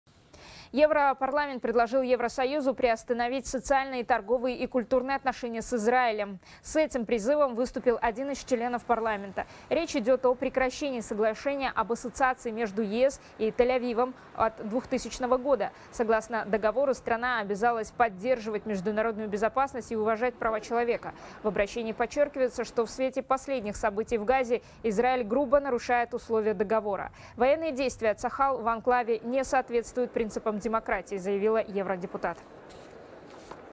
Соглашение между ЕС и Израилем призвали приостановить в ЕП
Европарламент предложил Евросоюзу приостановить социальные, торговые и культурные отношения с Израилем. С этим призывом выступил один из членов парламента, сообщает «24KZ».